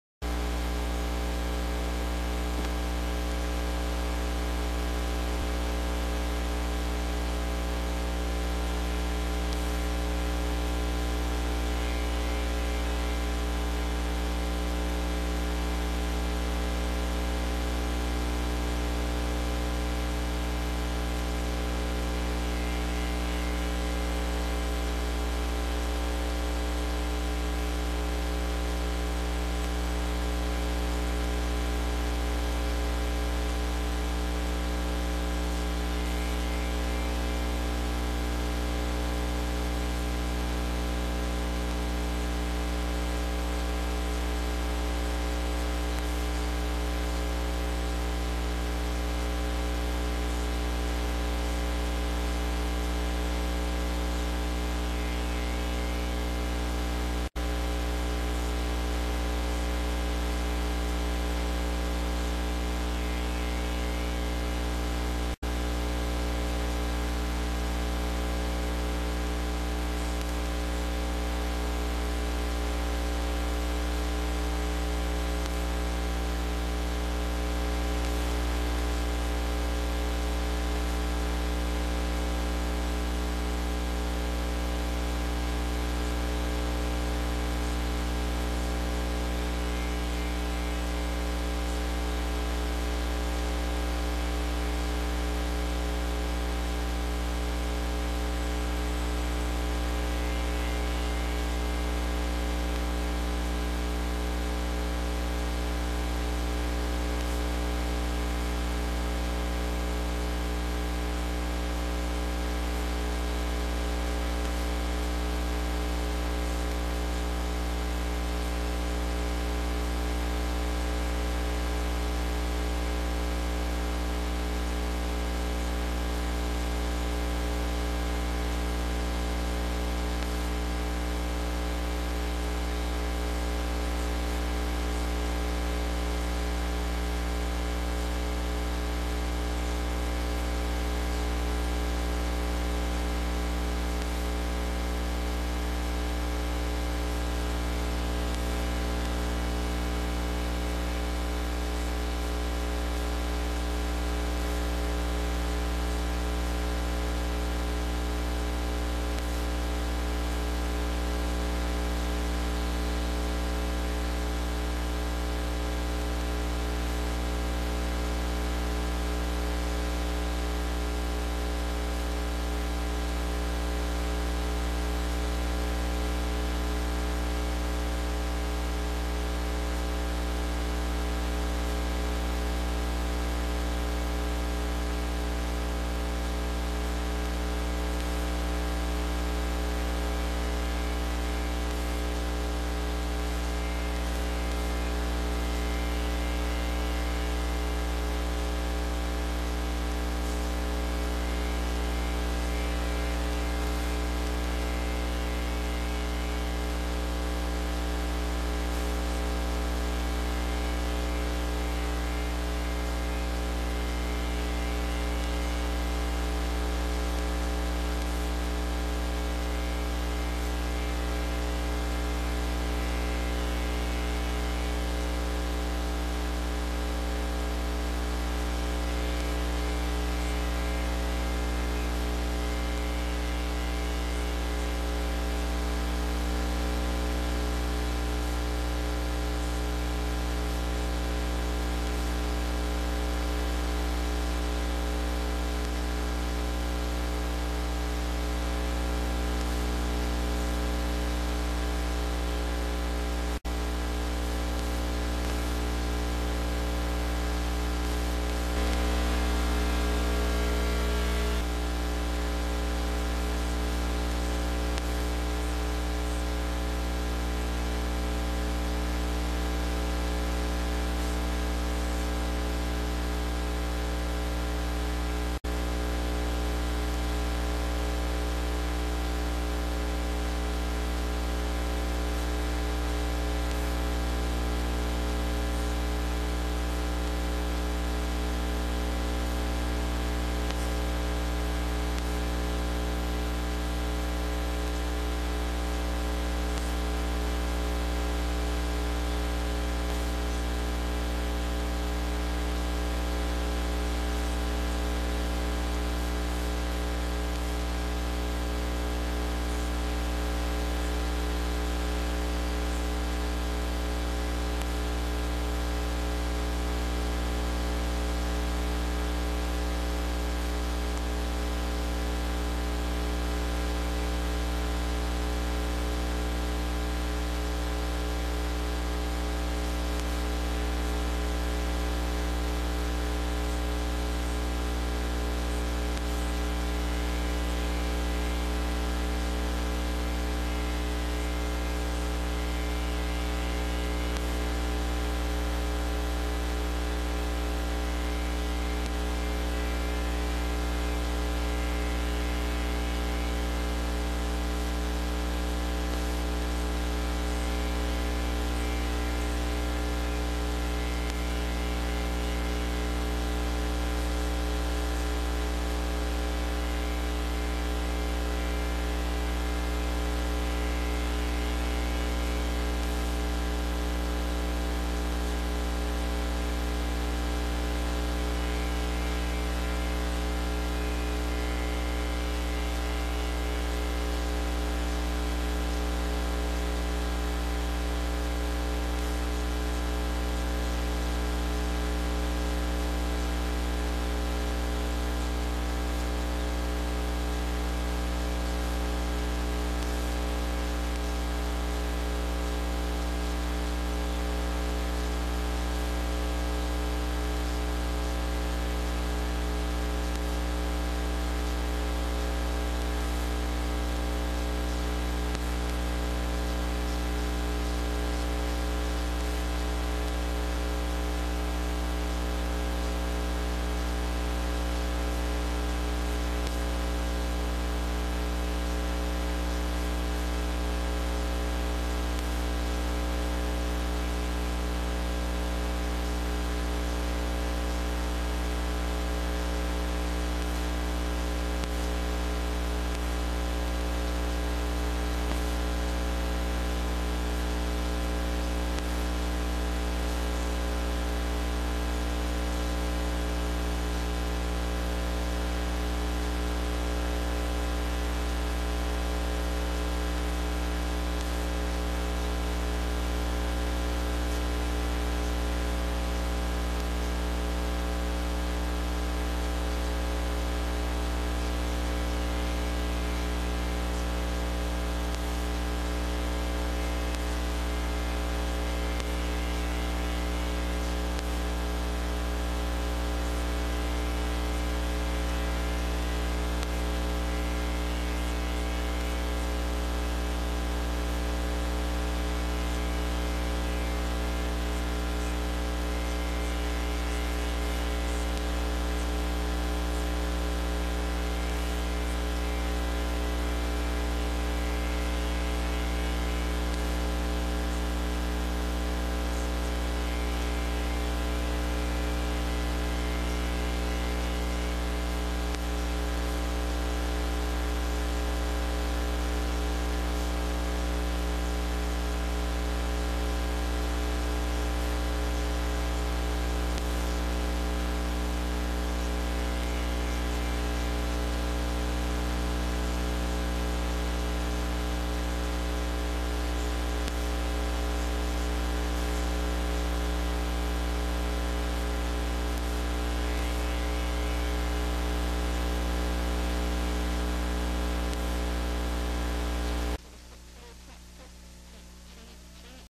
Guardian Footage
From Guardian - The Special Edition. Extra footage of locations in the QC that are gone like the Sportsland buidling, cars that we loved and still miss, and goofing around in a ravine with a camera.